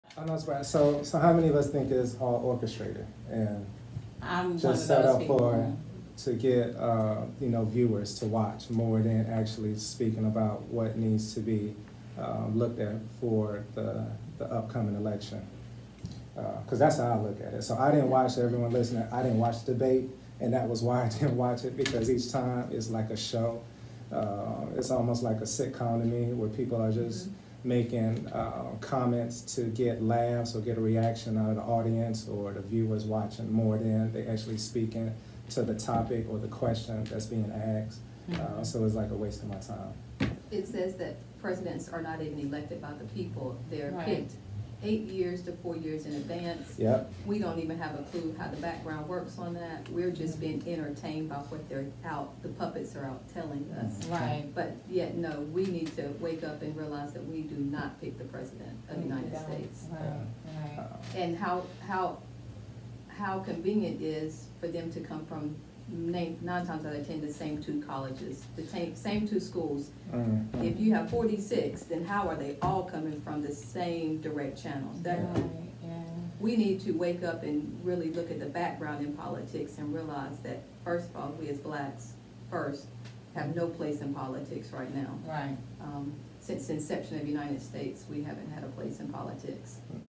Is the election orchestrated or do our votes count
Courtney joins Broadbelt SpeaX with Random Thoughts on presidential  election, solution for Black Americans and so much  more !